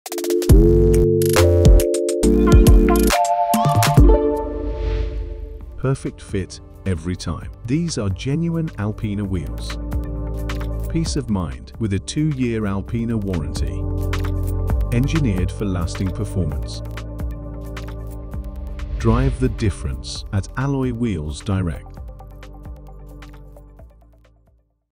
21" Alpina Classic Forged CS16 Wheels
Take a look at our 360 video of these incredible 21" Alpina Classic Forged CS16 Wheels.

These are Genuine Alpina alloy wheels.

Featuring a Black finish, a 2 Year Alpina Warranty and available to ship worldwide.

Alpina part numbers: 3611824SW and 3611825SW

Technical specs:
Front Wheels: 8.5Jx21 ET25 (P/N 3611824SW)
Rear Wheels: 10Jx21 ET41 (P/N 3611825SW)